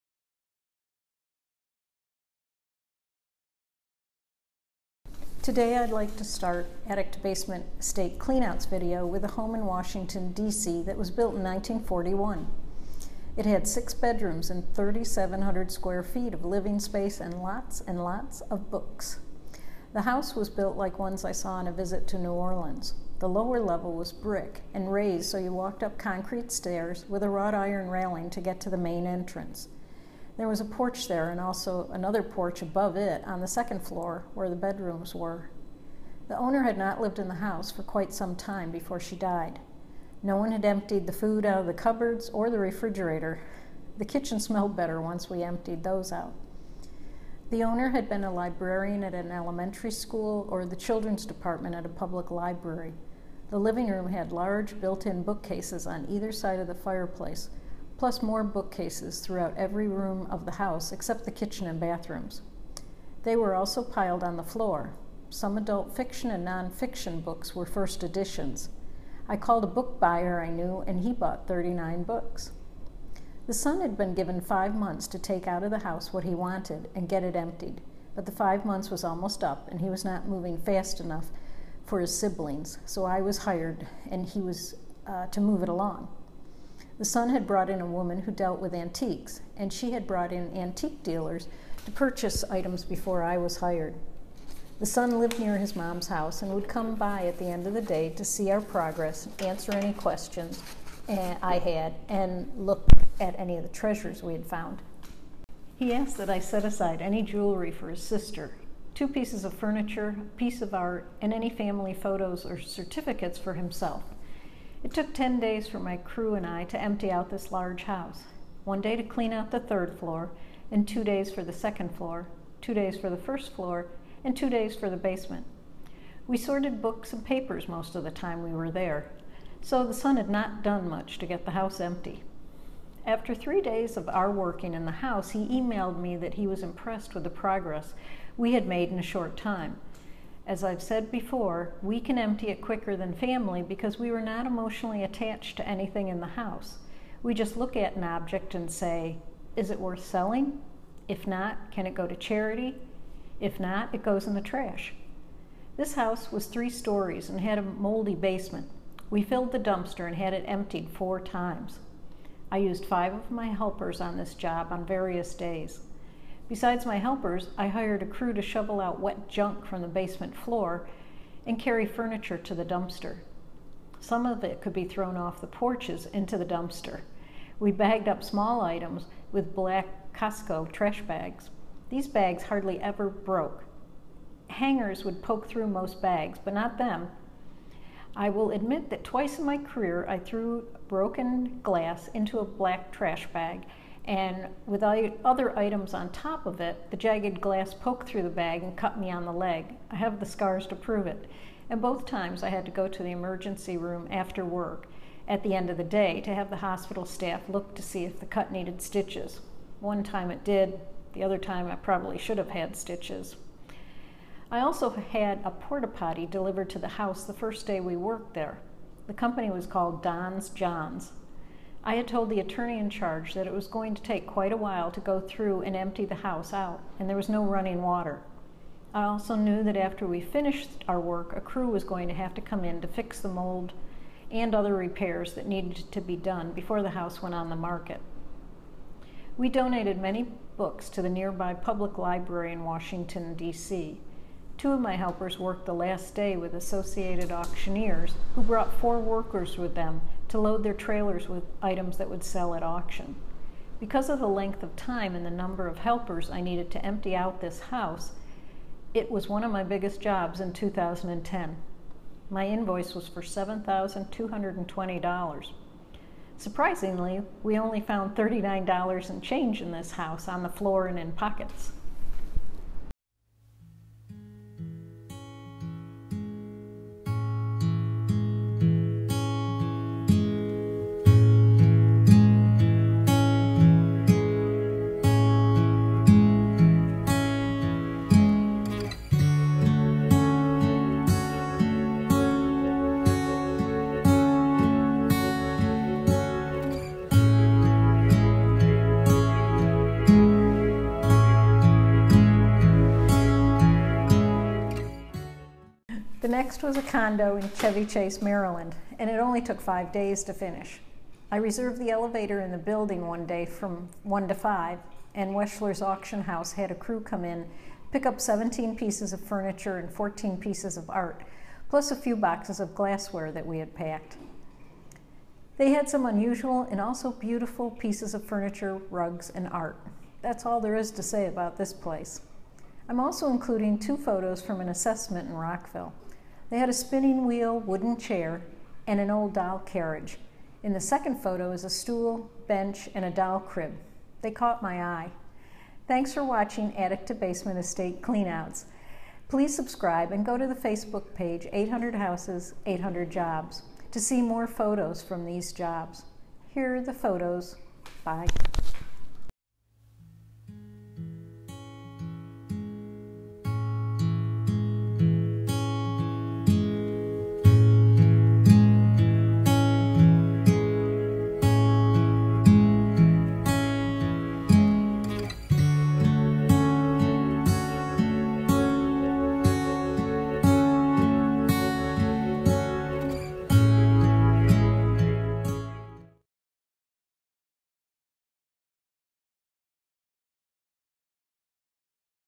Attic to Basement Estate Cleanouts with lots of BOOKS Episode 39
Today I have a house in NW Washington, DC that took 10 people and 10 days to empty.  At the end I threw in photos from a house with beautiful antique contents and art in a Chevy Chase, Maryland condo along with 2 photos from an assessment in Rockville, Maryland that had a chair made of spinning wheel parts and a doll crib and carriage.